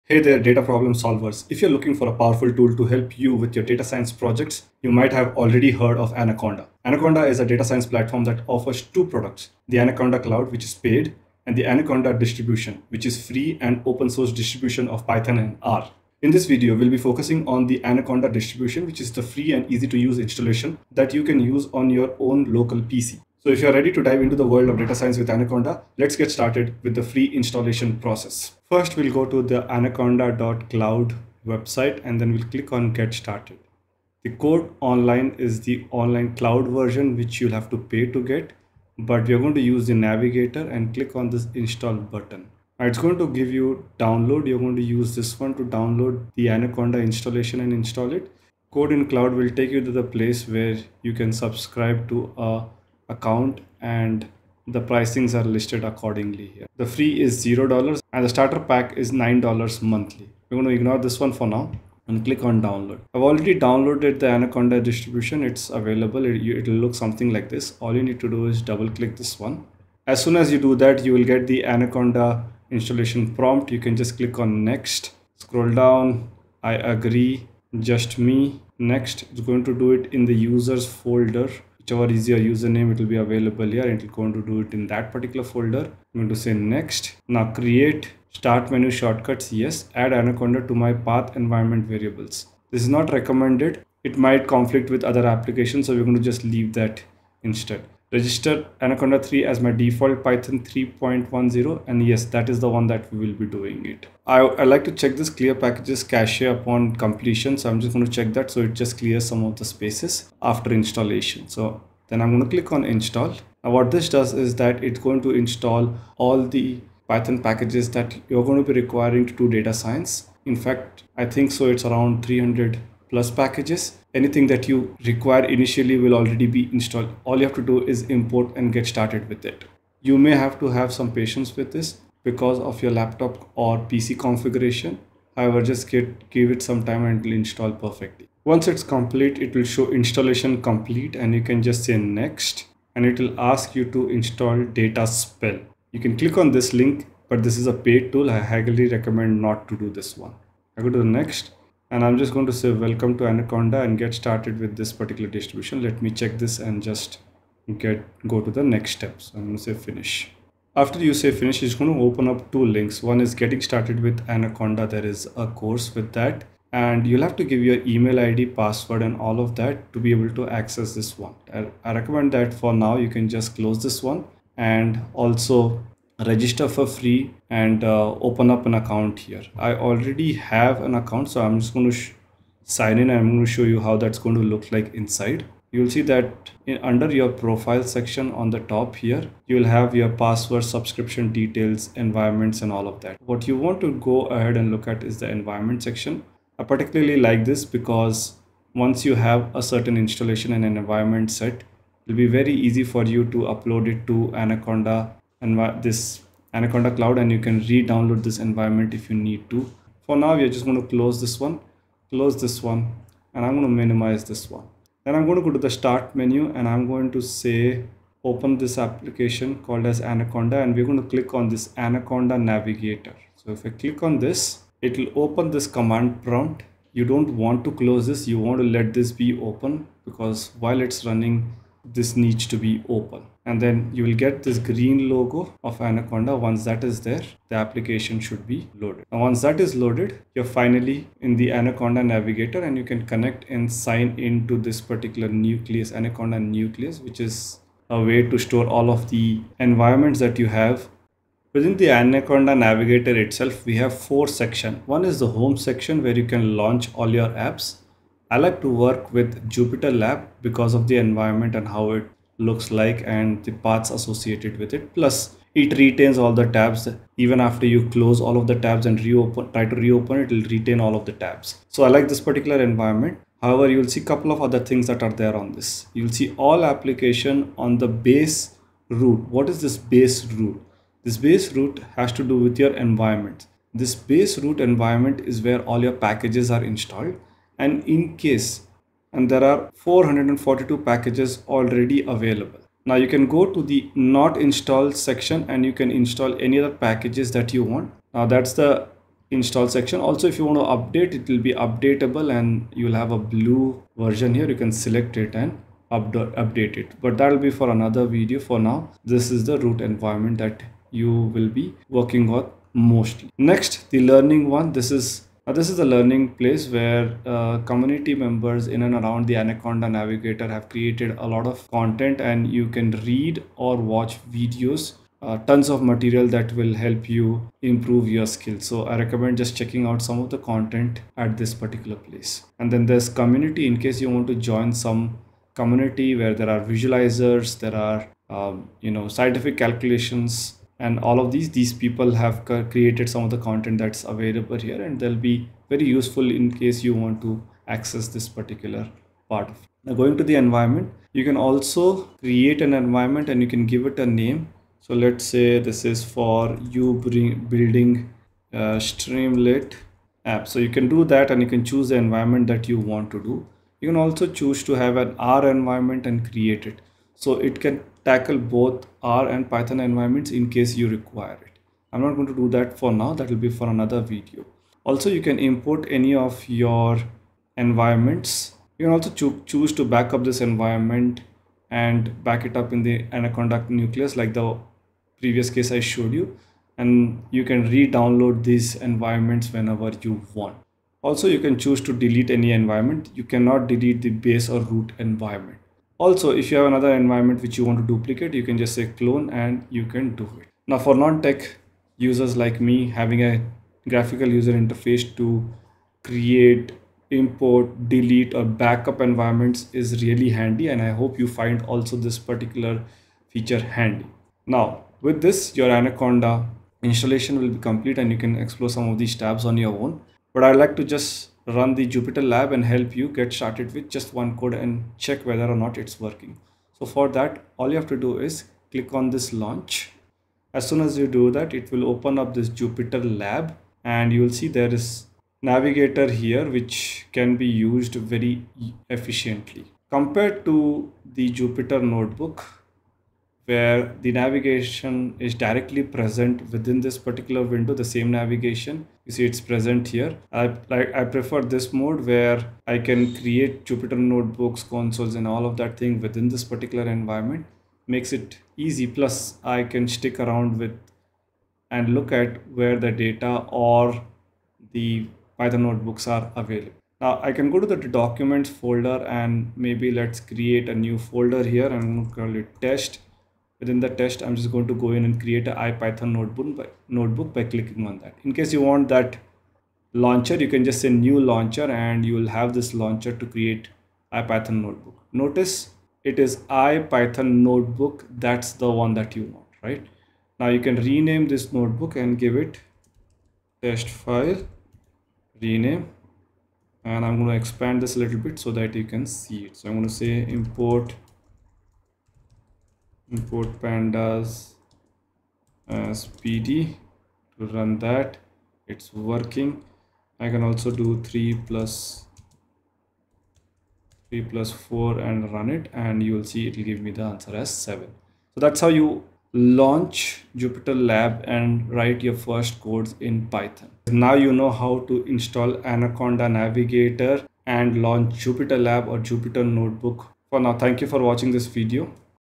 How to Install Anaconda?| Kunaal Naik
In this tutorial video, Kunaal Naik guides you on how to install Anaconda - the open-source distribution of Python and R programming languages used in data science. Whether you're a beginner or an experienced data scientist, installing Anaconda is a crucial step towards analyzing and visualizing your data. This video breaks down the installation process into easy-to-follow steps so that you can start working with Anaconda in no time.